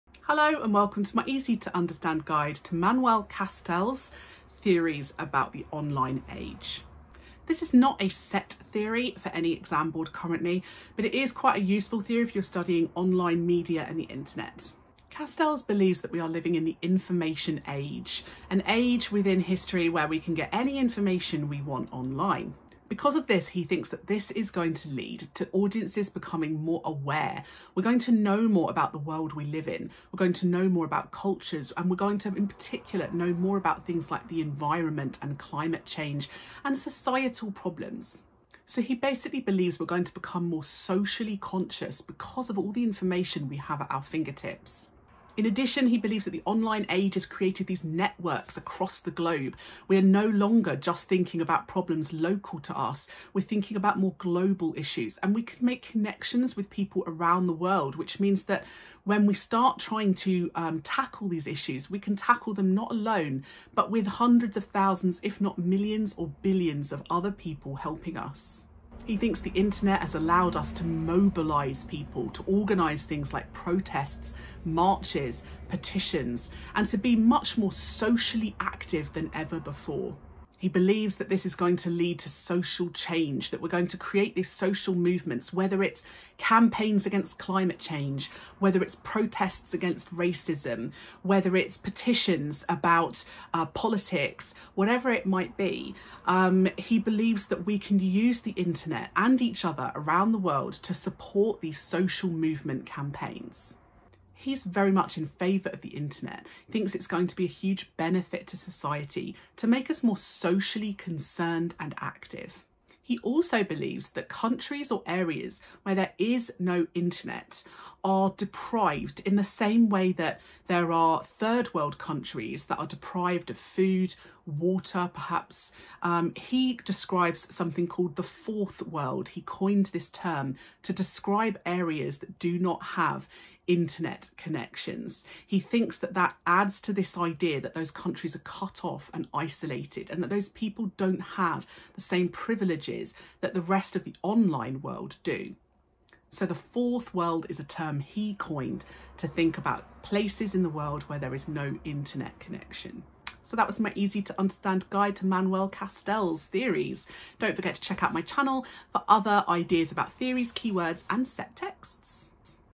Media Studies - Castell’s Theory of Online Media Age & Network Society - Simple Guide
Based on Manuel Castell’s ideas about the online Information Age and social activism online